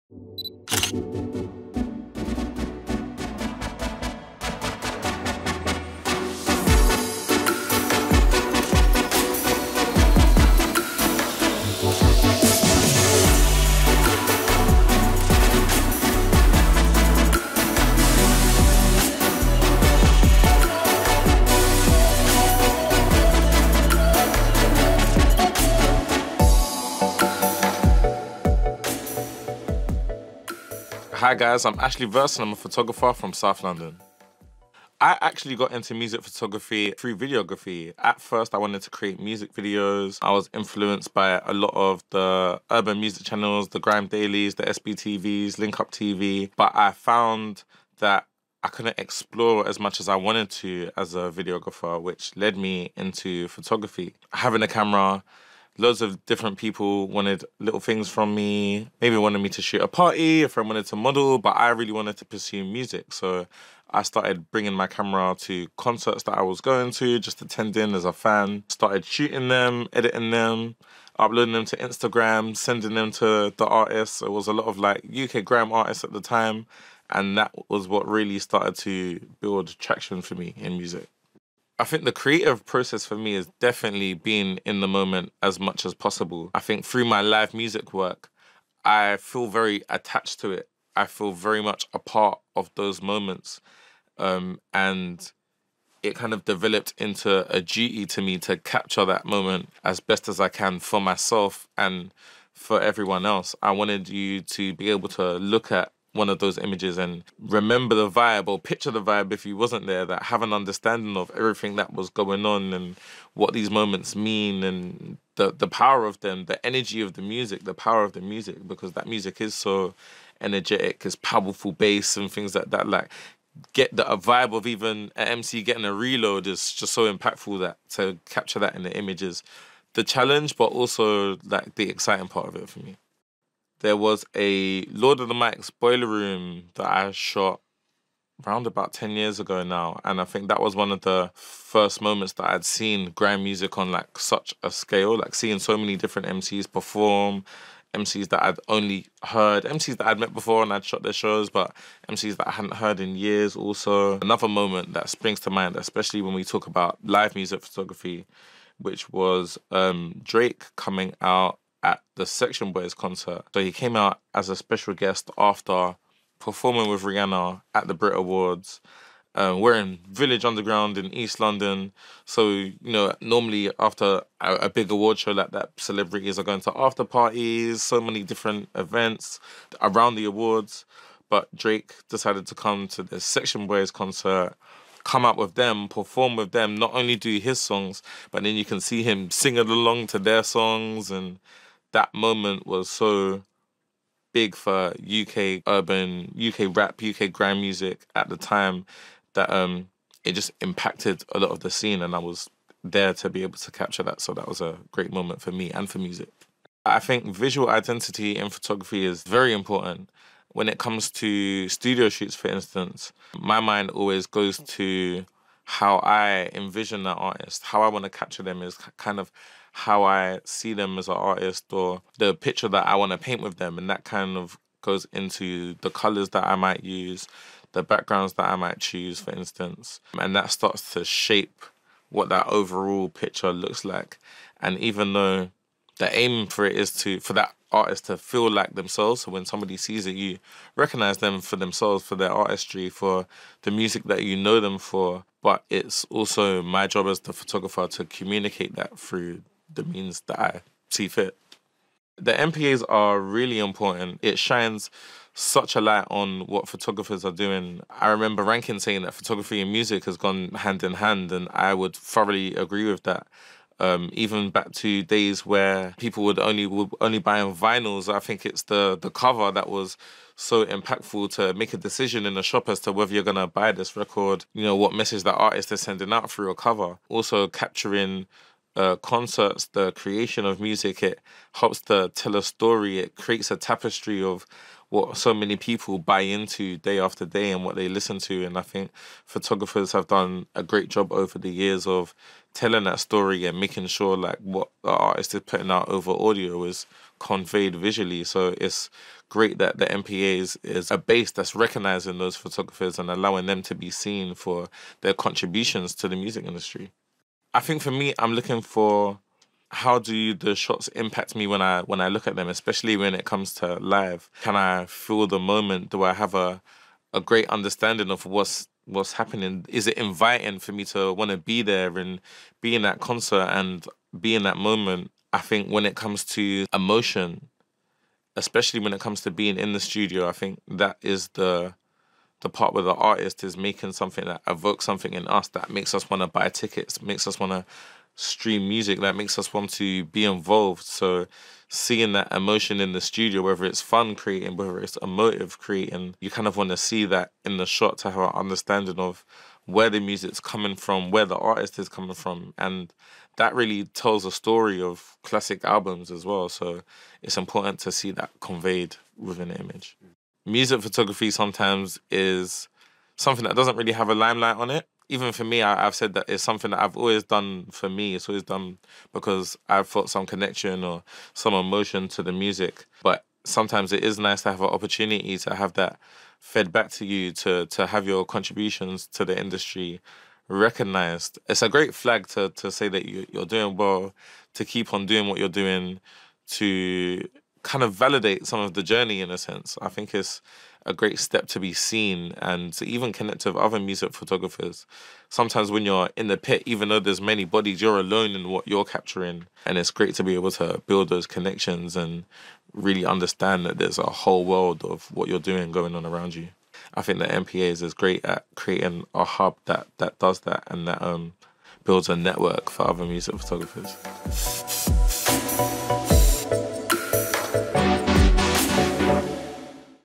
Meet the Judges: Ashley Verse
Ready for more? Well, here’s judge four - Ashley Verse

Ashley is a photographer of Jamaican & Bajan heritage, based in London. With years of creative exploration and dedication, Ashley Verse has become a respected name in the UK Music scene.

Recognised as a capturer of UK music culture, Ashley Verse has worked extensively with notable artists including Stormzy, Mabel & Tom Grennan, as well as working officially for entities such as MOBO, Wireless Festival & BET. With the constant urge to ‘explore’, his work has developed from capturing the moment to creating it, especially within his portraiture. His adaptable style has allowed him to make a true impact in his field, making him an influential figure amongst his counterparts.

Ashley wants to see your music images! Enter them for free now: mpa.photo.